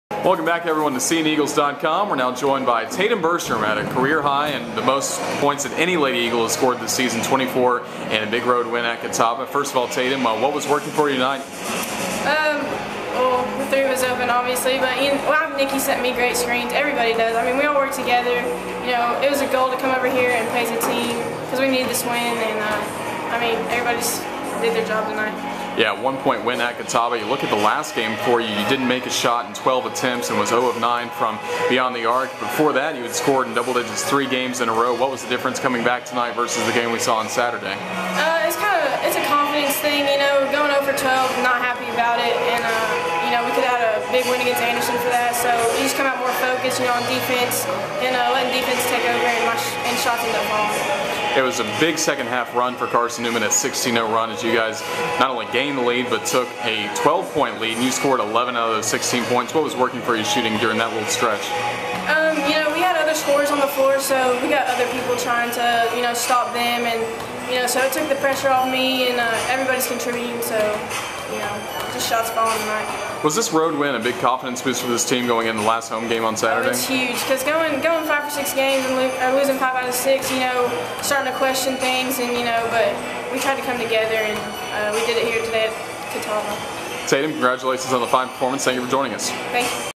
Carson-Newman Basketball: Tatum Burstrom Post Catawba 2-27-13
Carson-Newman's Tatum Burstrom visits with cneagles.com after the Lady Eagles beat Catawba 60-59 on Feb. 27, 2013.